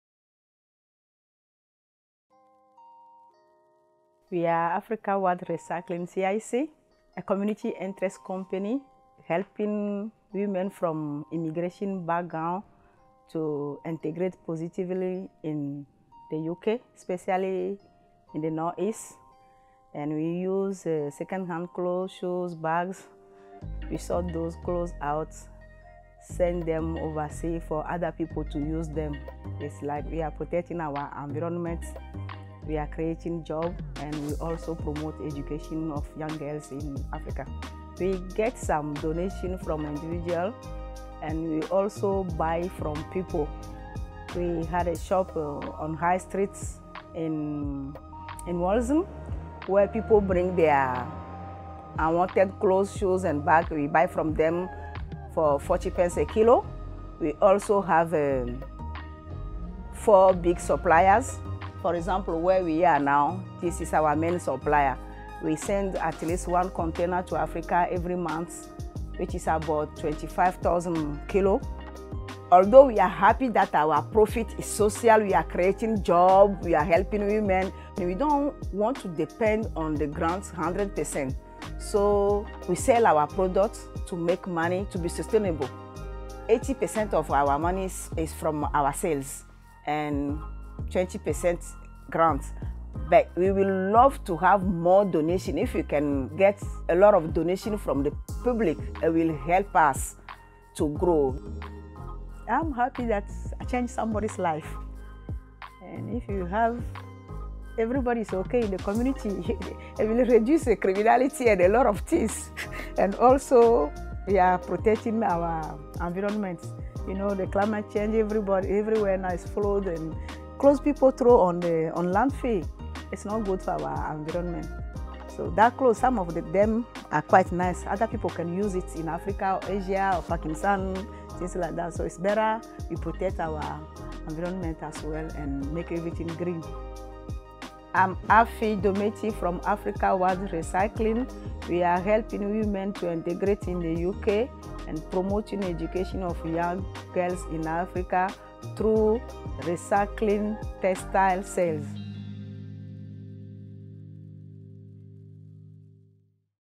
Social Enterprise Spotlight: Africaward Recycling CIC
Africawad Recycling CIC’s primary purpose is to support women. The activities of Africawad creates jobs, promotes education and reduces landfill waste both in the North East and globally.